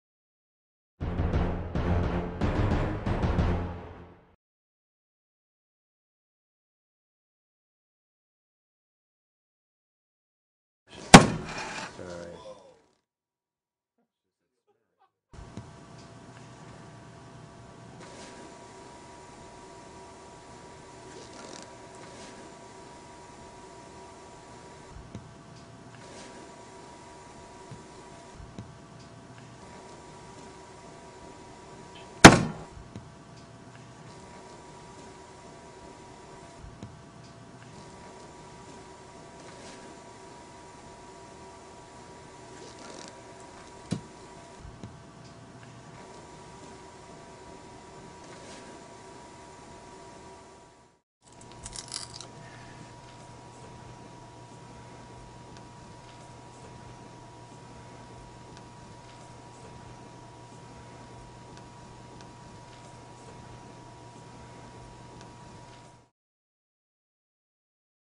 Solid Zirconia Crown vs PFM by Glidewell Laboratory
Hammer Test: Unconventional way to test Solid Zirconia Crown vs PFM